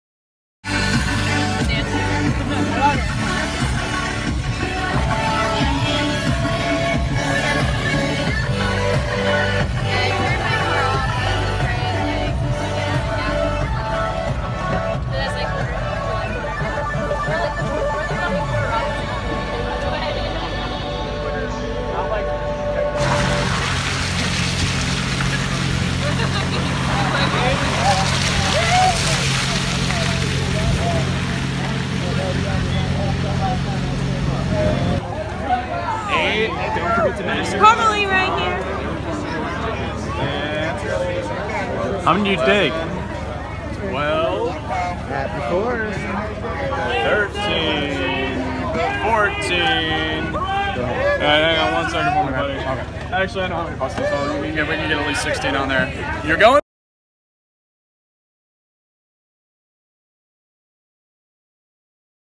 Rain and lightning delay Electric Forest campers
Festival-goers arrive for weekend 2 of Electric Forest. Many were delayed entering their campsites and leaving the pool party due to severe weather that lasted until about 4 a.m. on Wednesday, June 28, 2017 in Rothbury, Mich.